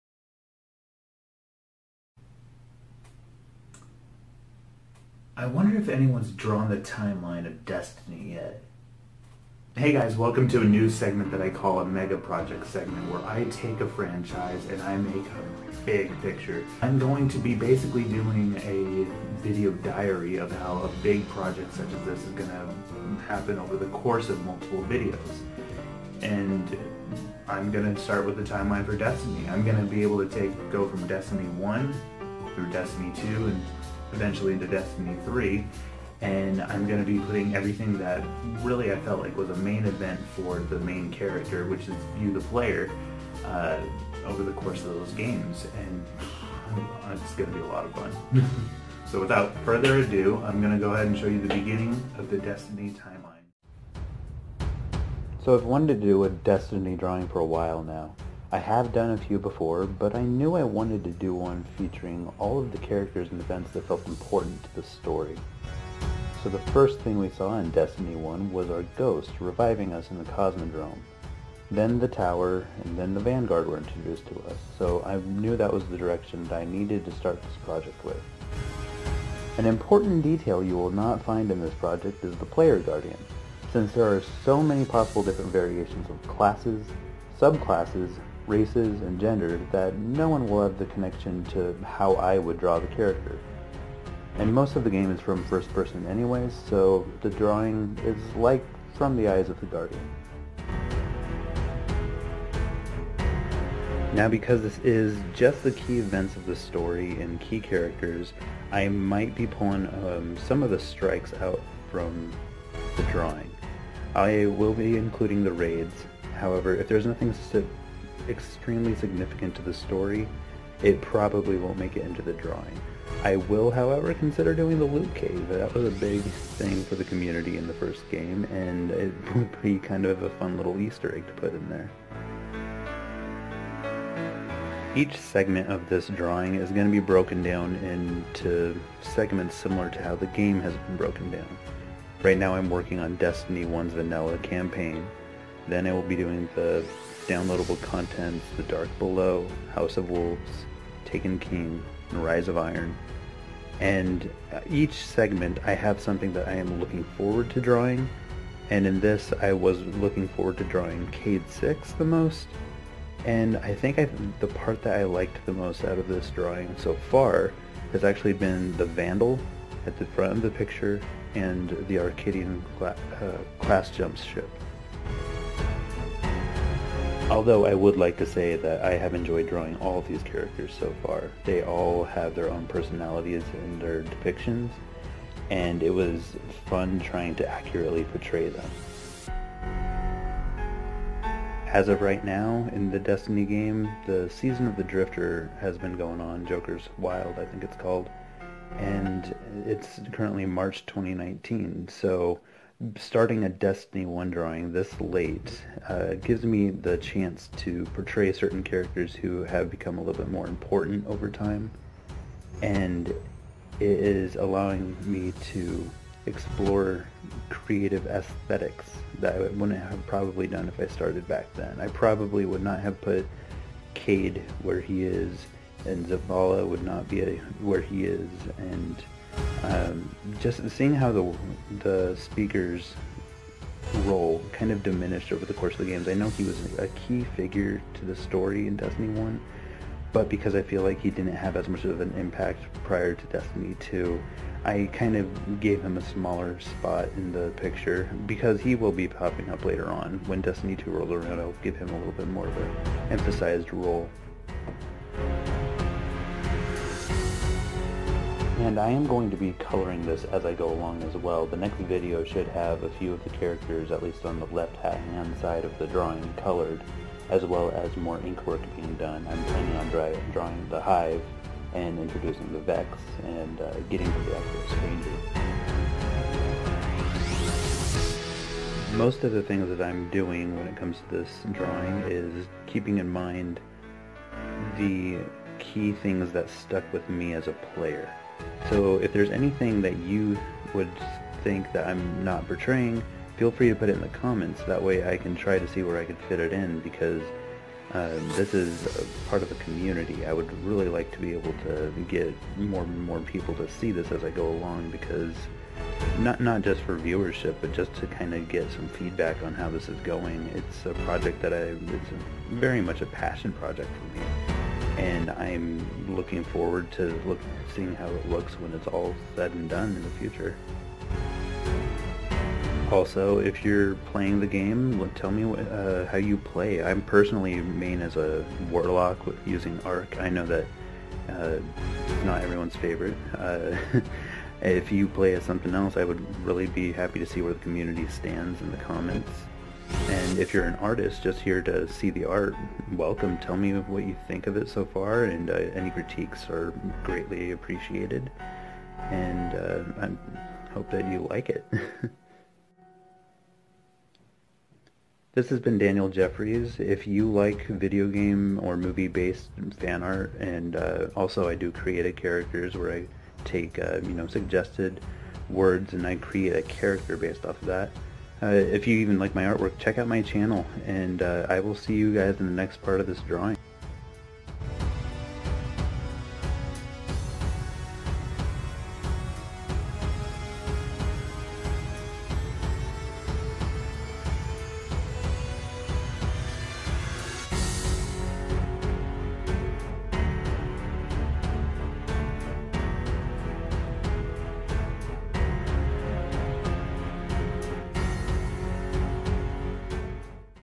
Drawing the Destiny timeline: Part 1
A time lapse video of me drawing a large ongoing mural of the main characters and events of the video game Destiny.